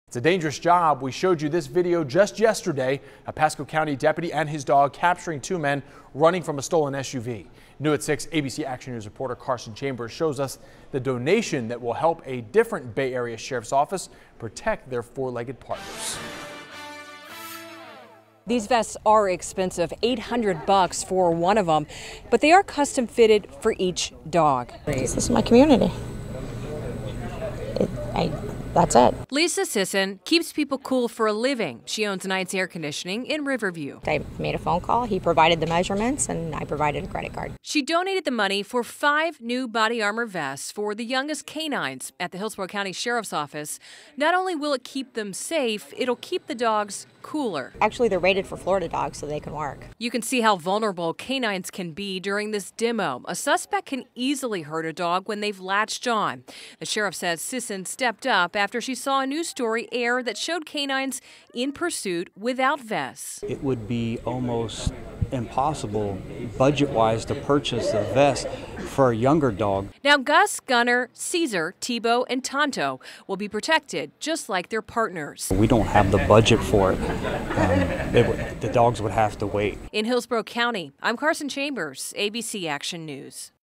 Donor gives $4,500 in K9 body armor to Hillsborough County Sheriff's Office
A local woman and her company have donated five complete sets of body armor to the Hillsborough County Sheriff's Office K9 unit.